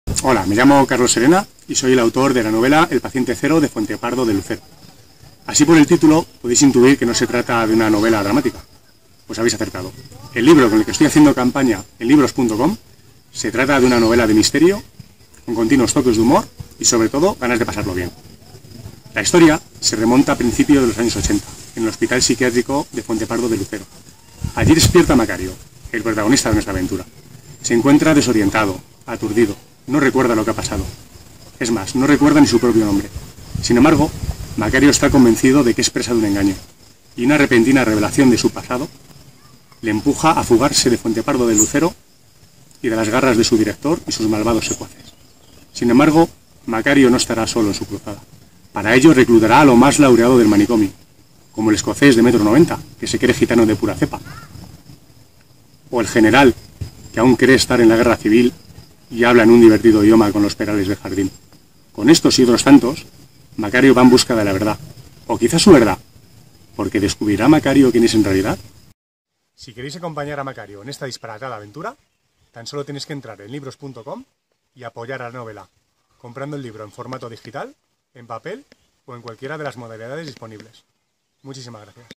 El paciente 0 - Carlos Serena Ramos (Editorial Libros.com, 2015)
Principios de los años ochenta, Macario Pedregales despierta en Fuente Pardo del Lucero, un hospital psiquiátrico que comparte nombre con un pequeño pueblo de la provincia de Zamora. Se encuentra esposado a la cama, aturdido y mareado. Se siente extraño, quizá por el hecho de no recordar nada de lo que ha sucedido, es más, por no recordar nada de su pasado; ni siquiera su propio nombre. Sin embargo, Macario está convencido de que es víctima de un engaño. Algo en su interior le impide creer las palabras del director, hasta que una misteriosa y repentina revelación de su pasado le empuja a tomar la firme decisión de fugarse de Fuente Pardo del Lucero e ir en busca de la mujer e hija que aparecen en sus sueños. Pero no estará solo en su cruzada, para ello reclutará a lo mejorcito y más laureado del manicomio: un elenco de singulares y extravagantes compañeros, como el rubio escocés de metro noventa que se cree gitano de pura cepa, o el coronel jubilado que aún cree estar en la guerra civil y habla con los perales del jardín. Con ellos y otros tantos, vivirá infinidad de situaciones disparatadas intentando averiguar la verdad. ¿Descubrirá Macario quién es en realidad?